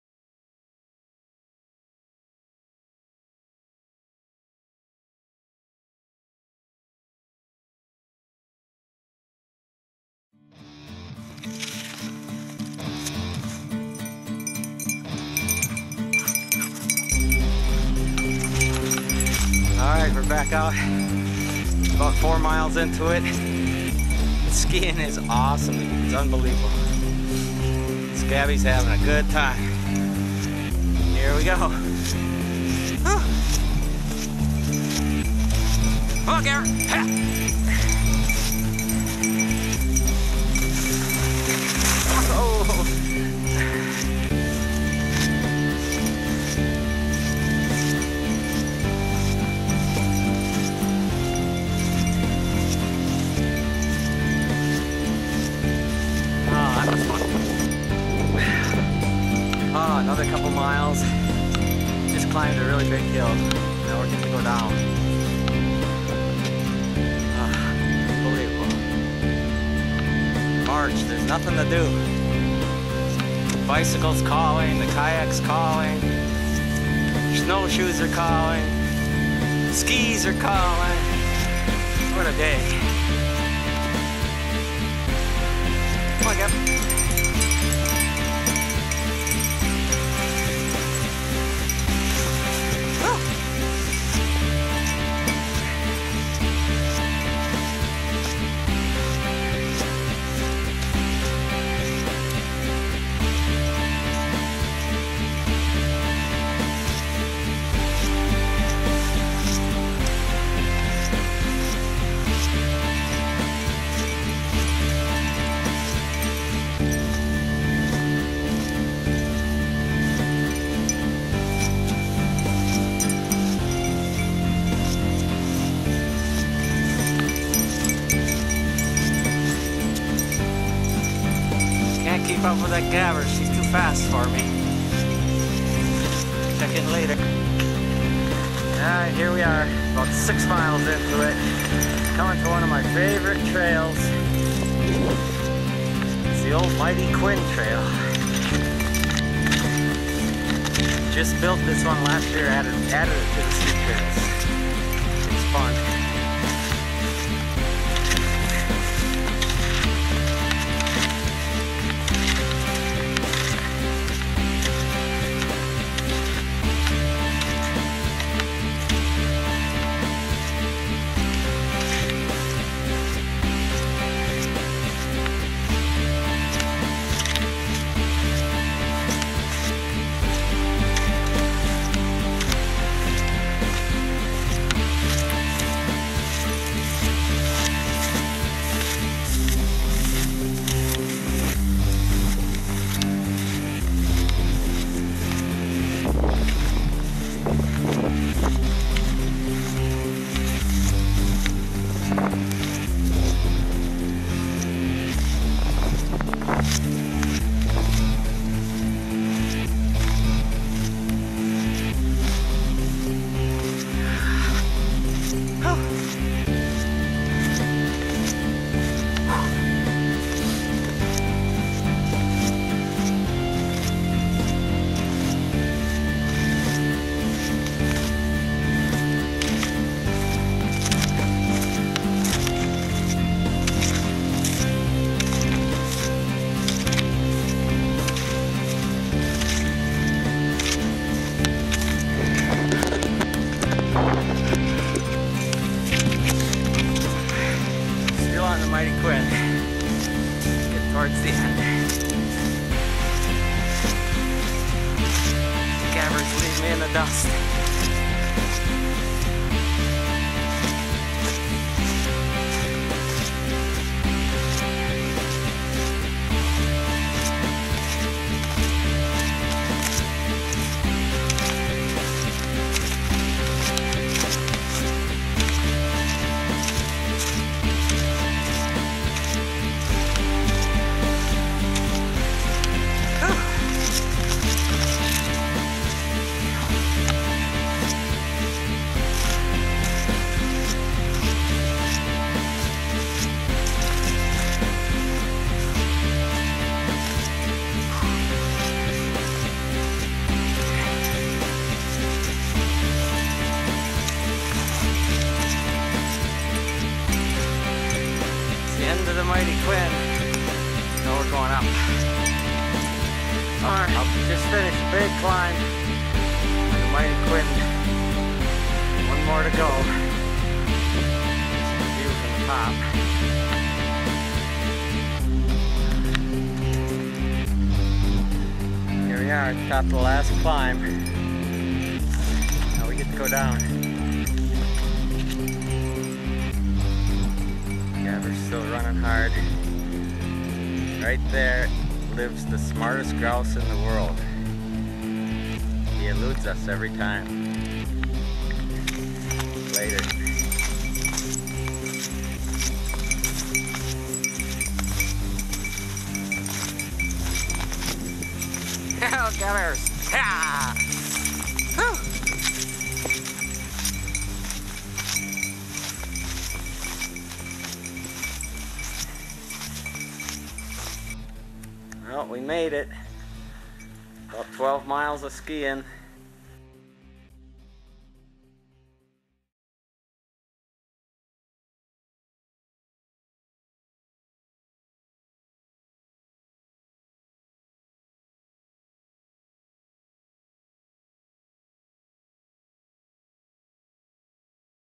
Skiing at Razorback Ridges
A Sayner, WI resident who voluntarily grooms the skate ski trail at Razorback Ridges all winter long, takes us on one of his journeys. He tries to keep up with his faithful companion, a German Shorthair Pointer named Gabby as he moves along the trail through the beautiful Northwoods.